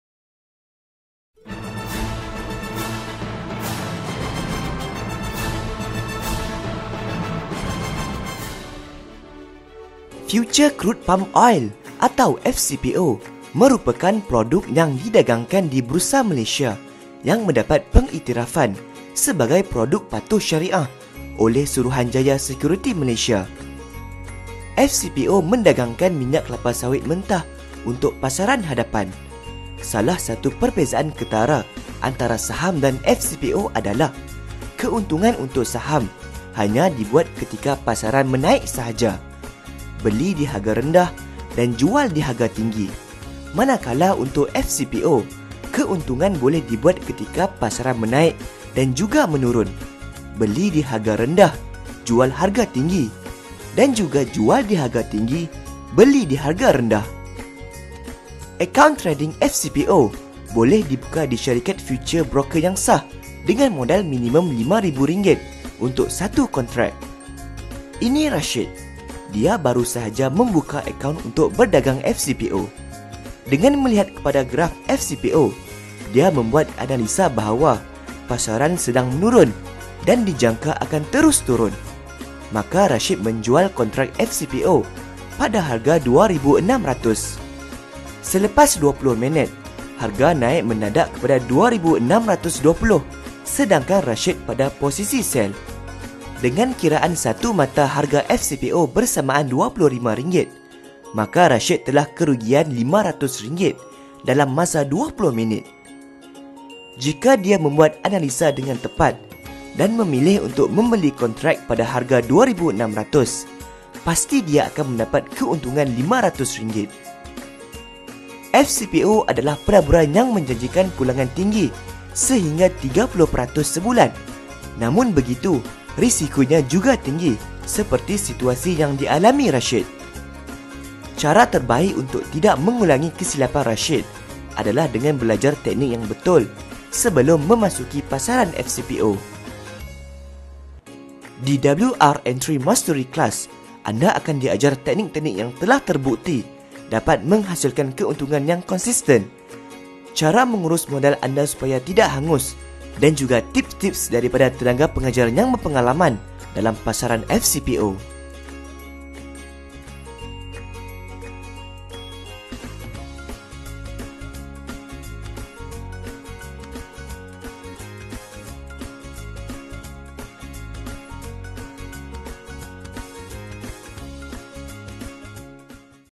Pengenalan Future Crude Palm Oil (FCPO)
Ini adalah asas Teknikal analisis dalam fcpo jika anda ingin tahu.
Ia diberi secara percuma dan anda boleh lihat video ini berulang ulang kali
Didalam video diberi idea analysis dan kalau anda seorang genius mungkin anda akan sukses selepas lihat video ini dan teknik asas ini membolehkan anda menjana 30-50 percent income sebulan InsyaAllah.

terima kasih!